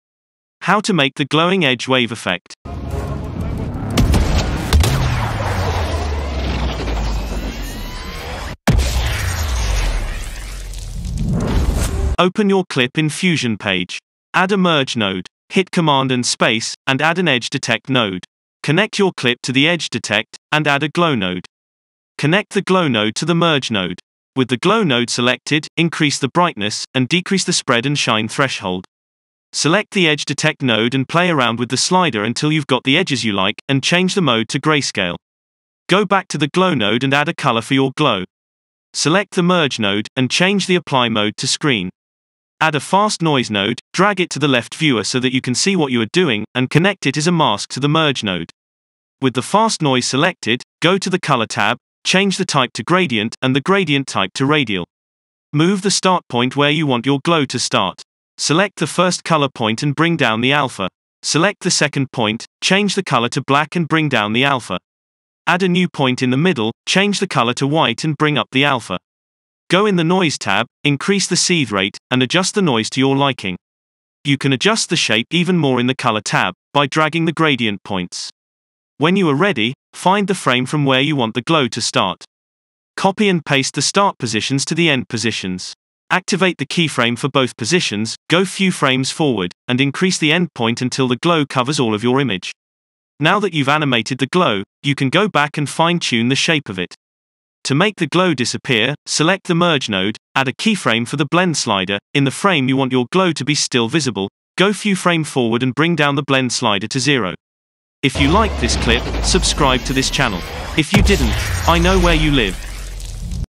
How To Make The Glowing Edges Wave Effect | DaVinci Resolve 17
How to make the glowing edges wave effect ( frogling edit ) in davinci resolve 17